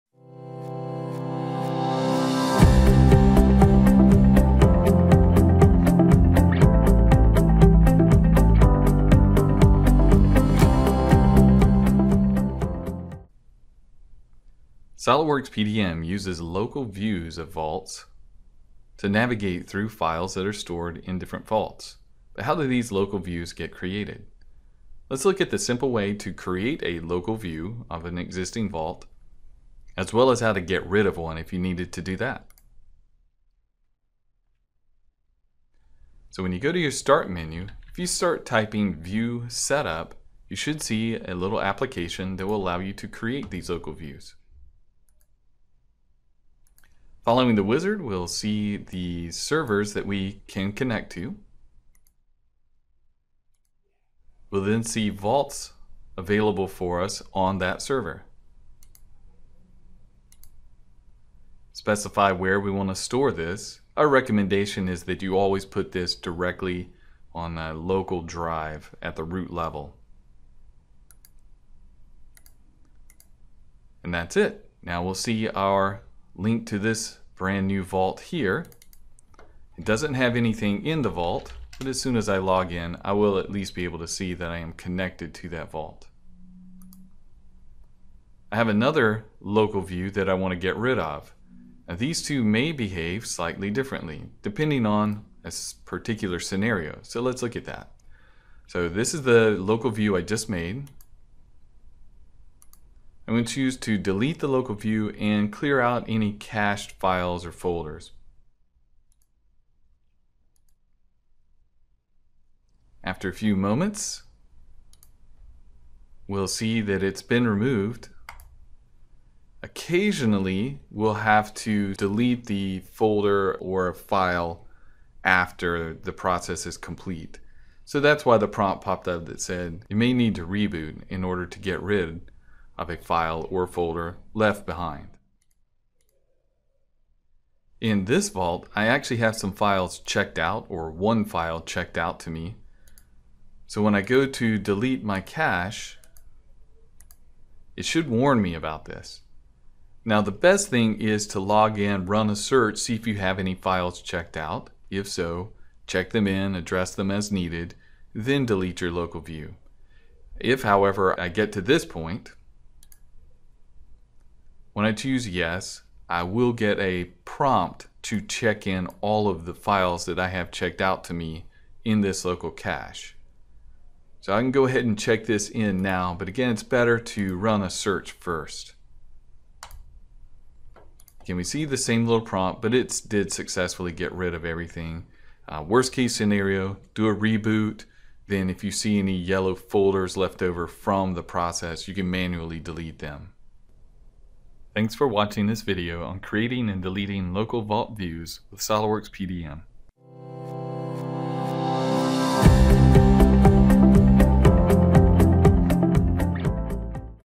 Video Tech Tip: Create & Delete Local Vault Views in SOLIDWORKS PDM
Quickly navigate to files stored on different vaults using local vault views in SOLIDWORKS PDM. We'll show you how to set up local vault views and get rid of ones you don't need.